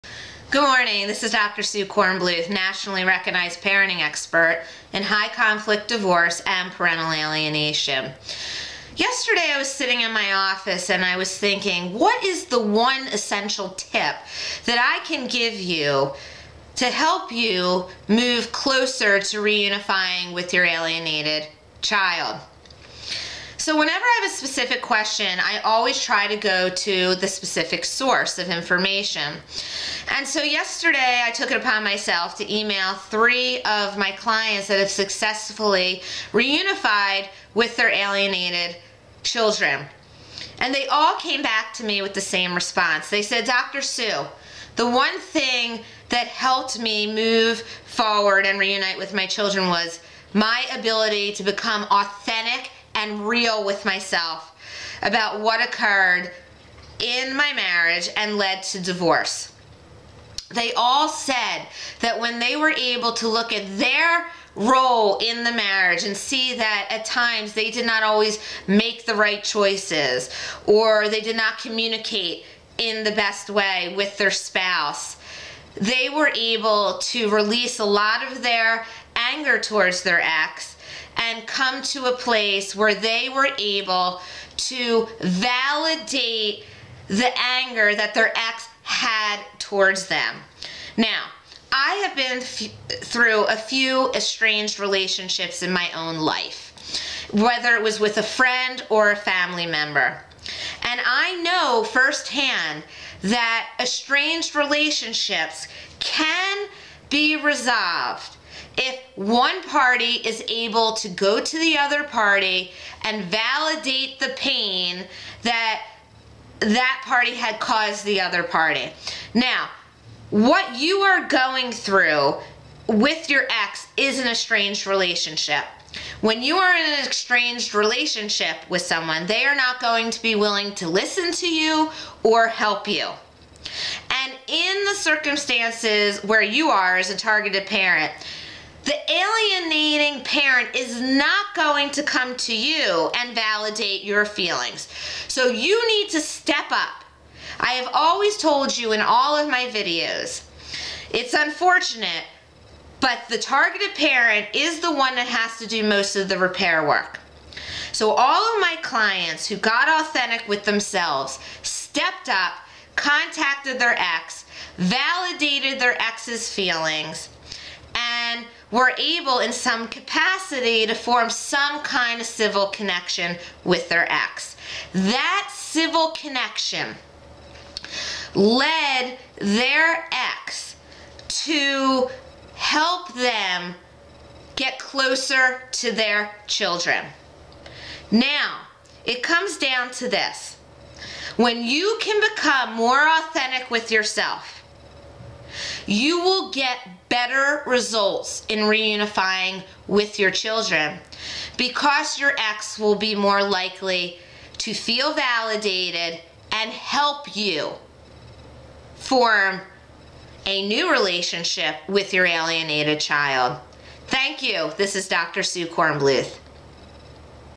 How authenticity can change your parent alienation situation?|Dr. Sue Cornbluth
Dr. Sue Cornbluth, leading expert in high conflict divorce and parent alienation informs you about how becoming authentic with yourself improves your chances of reuniting with your children.